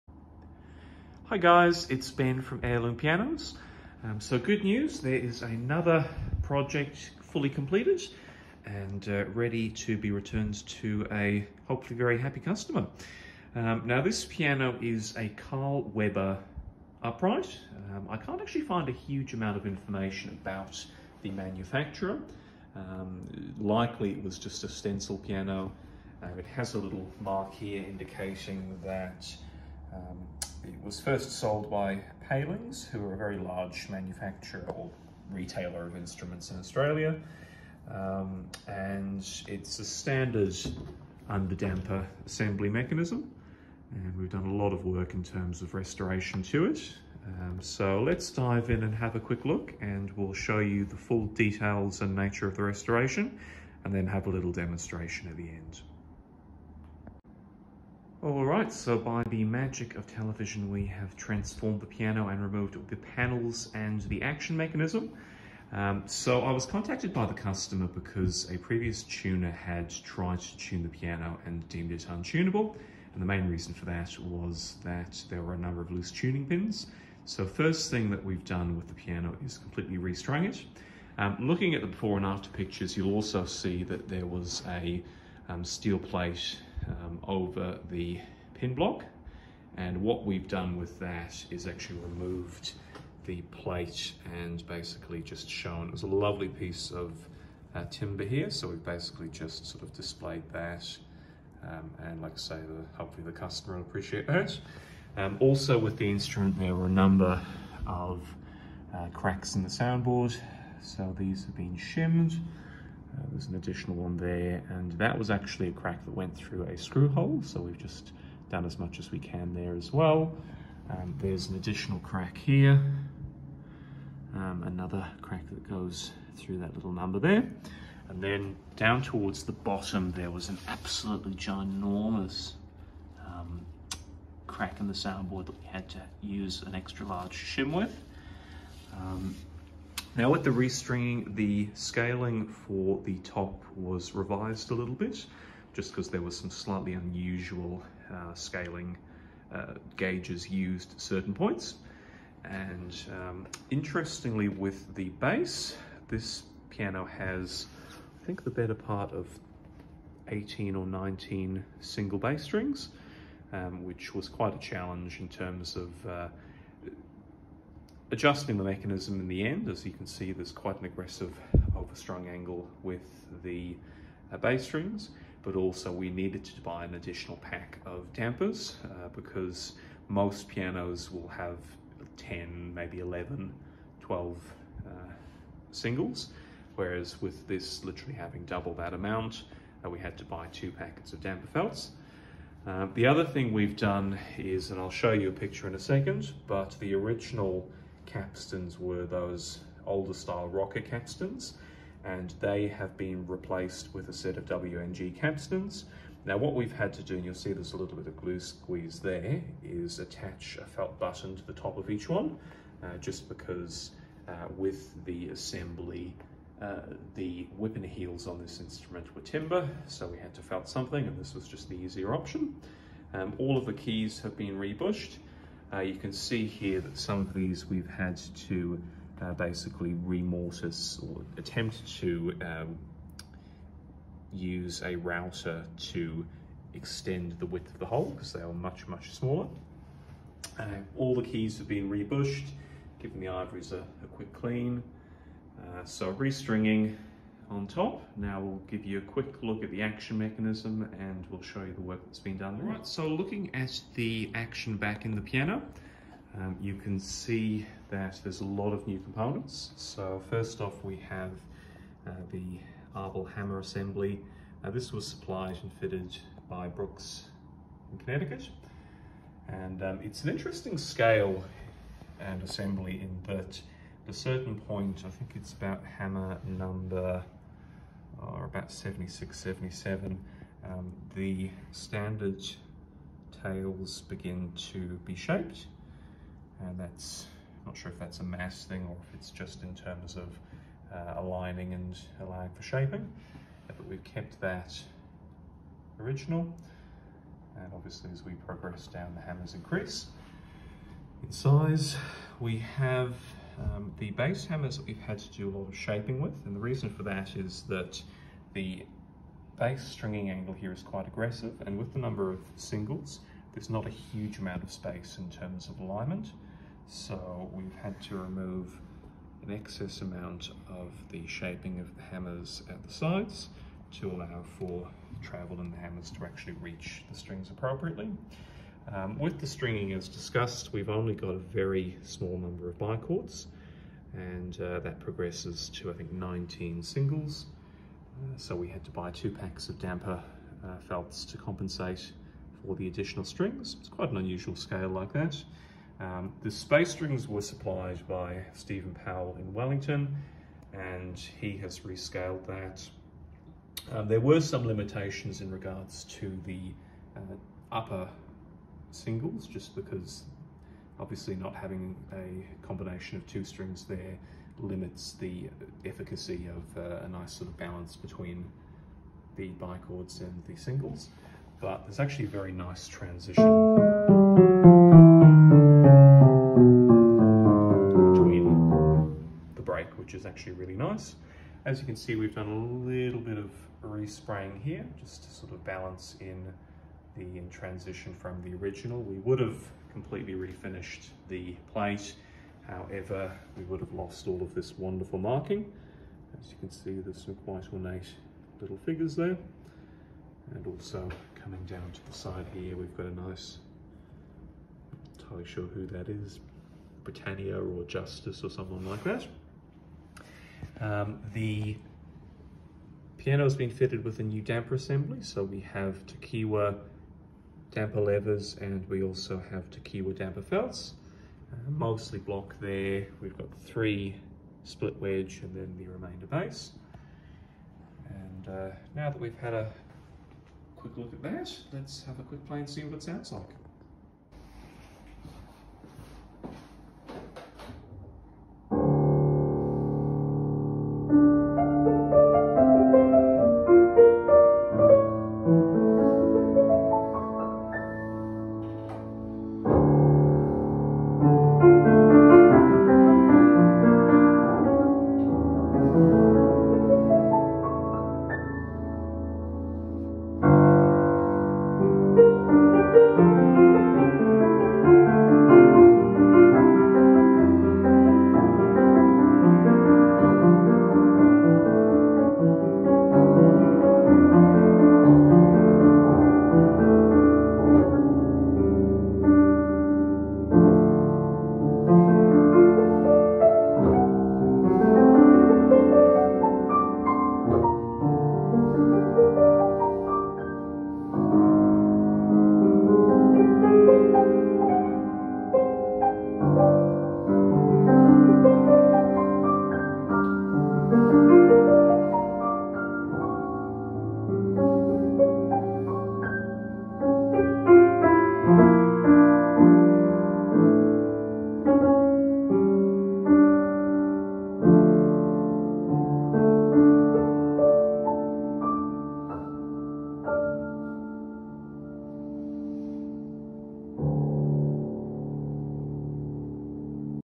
1912 Carl Weber piano restoration
In this video we detail the extensive restoration of the 1912 Carl Weber Piano.